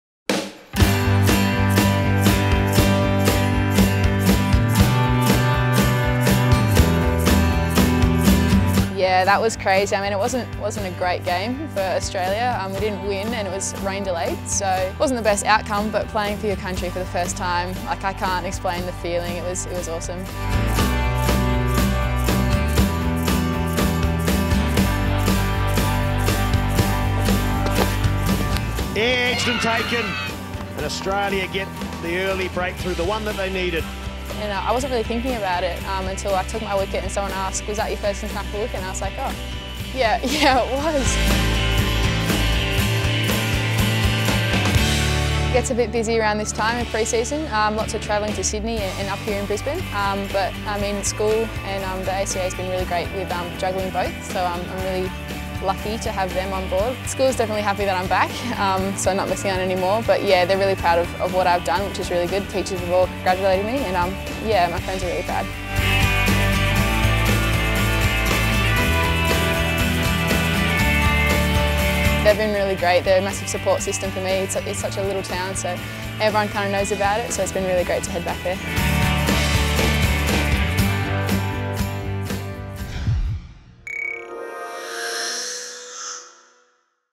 Get to know Lauren Cheatle
Teenage quick Lauren Cheatle - one of Australia's most exciting young players - chats about her international debut and juggling high school with cricket